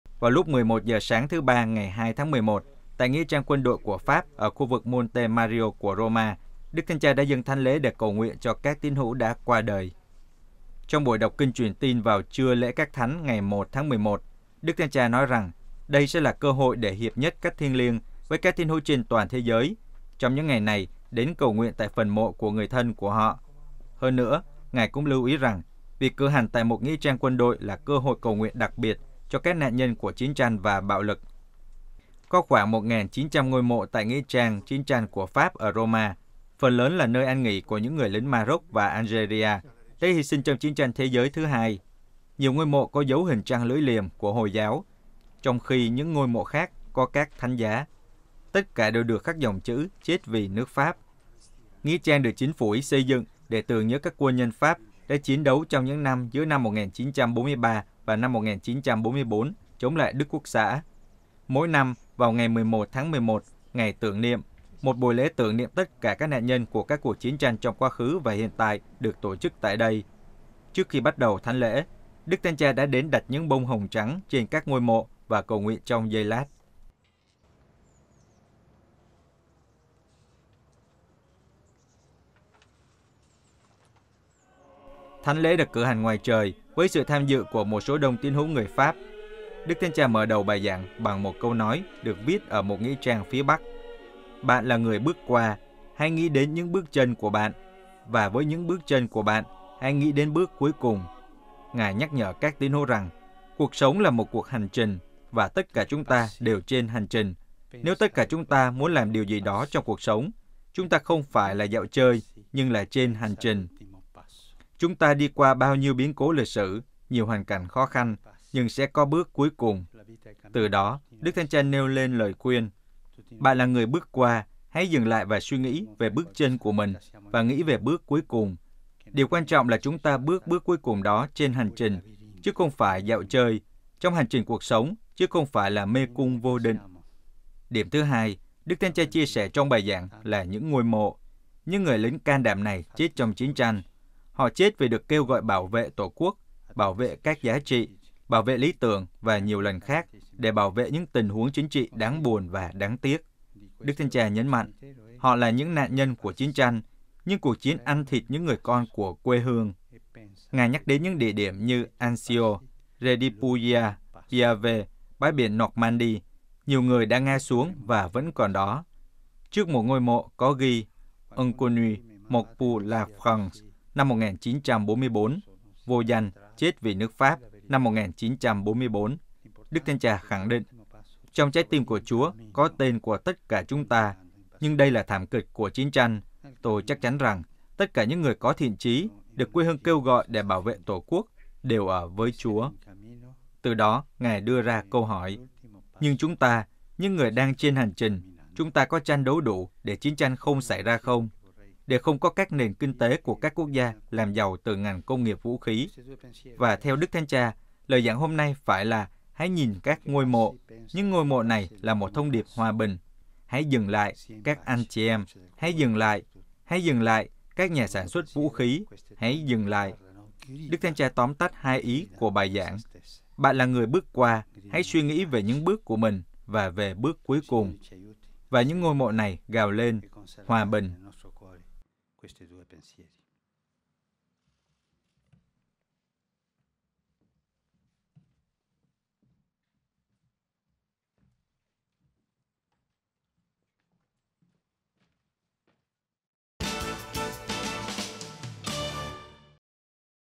ĐTC dâng Thánh lễ cầu nguyện cho các tín hữu đã qua đời
Vào lúc 11 giờ sáng thứ Ba ngày 2/11/2021 tại Nghĩa trang Quân đội của Pháp ở khu vực Monte Mario của Roma, Đức Thánh Cha đã dâng Thánh lễ để cầu nguyện cho các tín hữu đã qua đời.